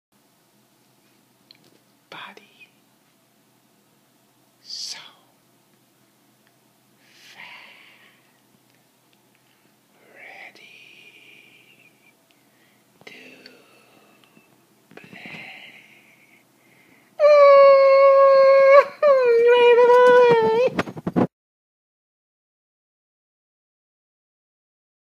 Ready to Play?
Prepping Yoshi for her morning walk.